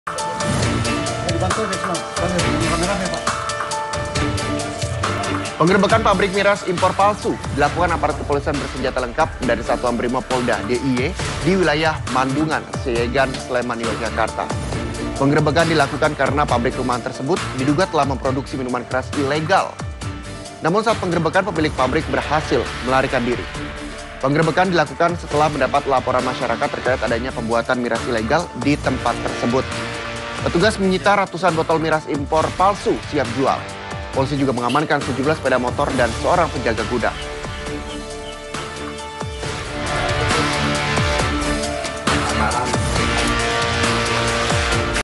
Pabrik Miras Impor Palsu Digerebek
pabrik miras di kawasan sleman yogya ini digerebek aparat kepolisian kerena terbukti memproduksi miras palsu dengan berbagai merk terkenal. namun sayang dalam pengerebekan ini pemilik pabrik berhasil kabur.